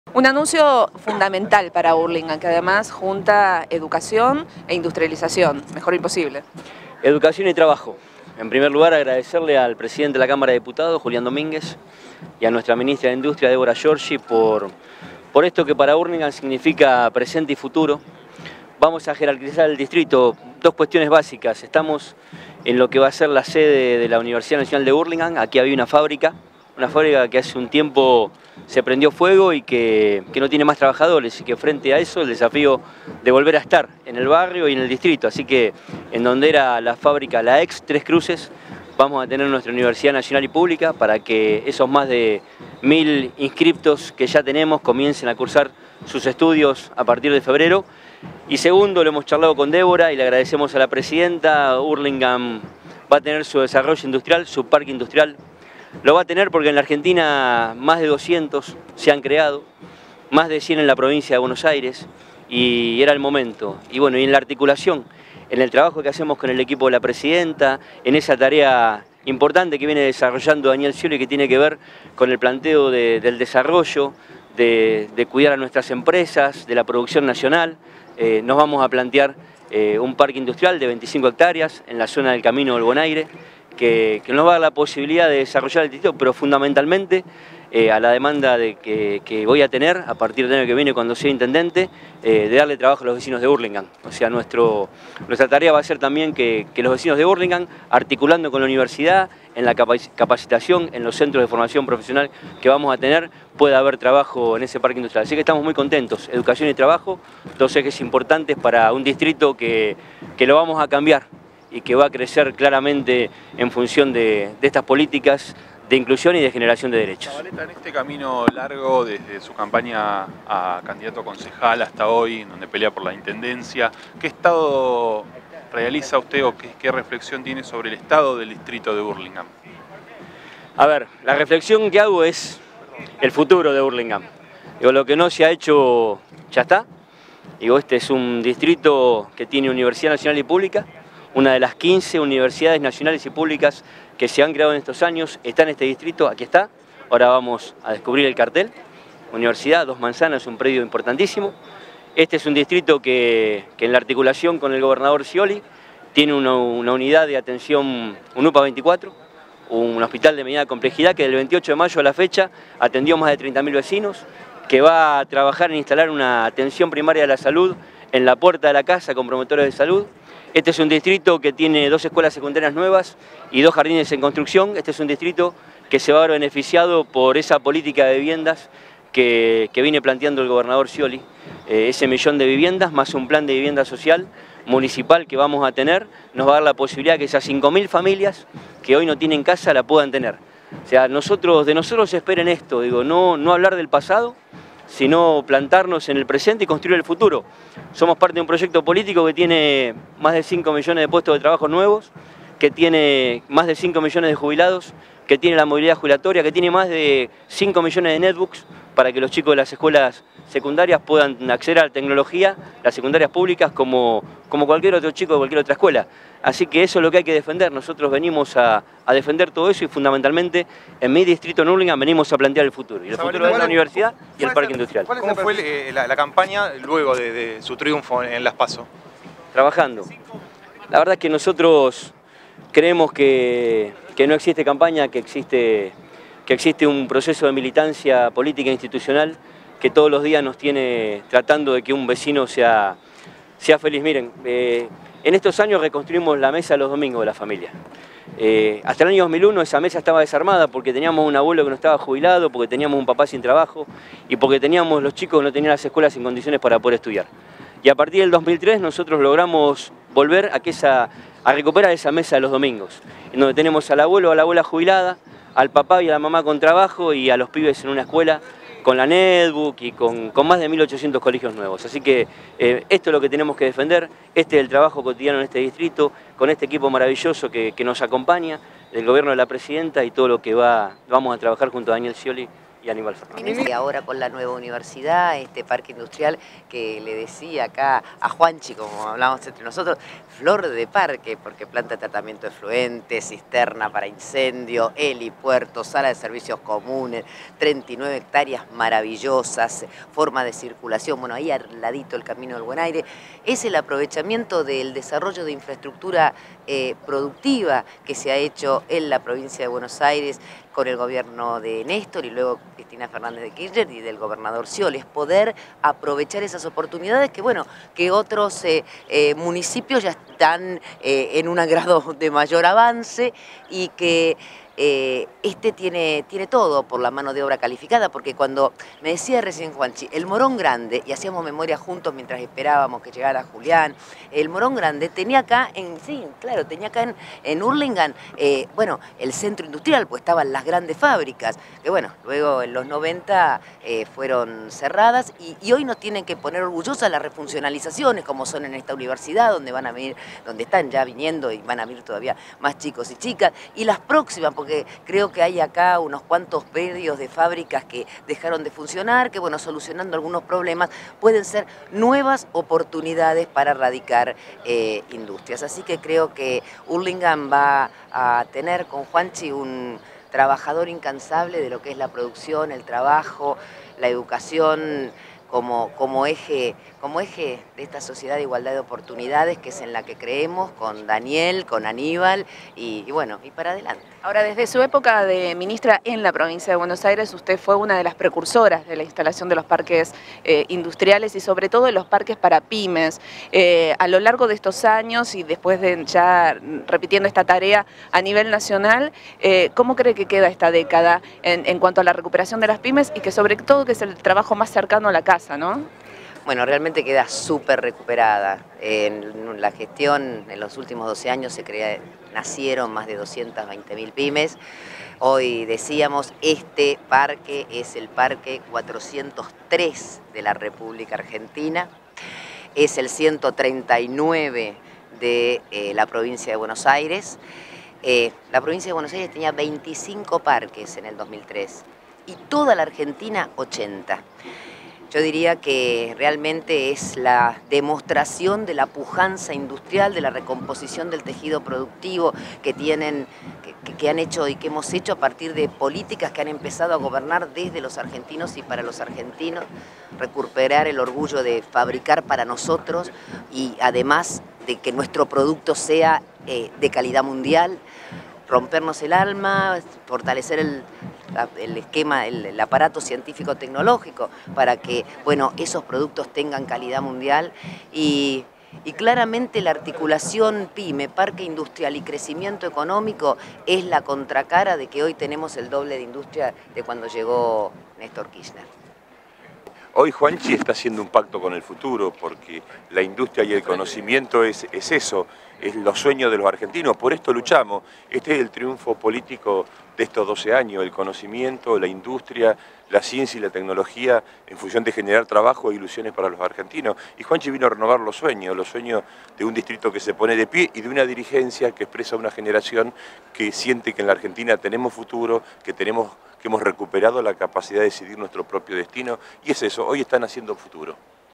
La Universidad de Hurlingham ya tiene edificio propio
Juan Zabaleta presentó el edificio donde funcionará la nueva Universidad Nacional de Hurlingham. Y anunció también el futuro Parque Industrial en tierras cedidas por el gobierno nacional.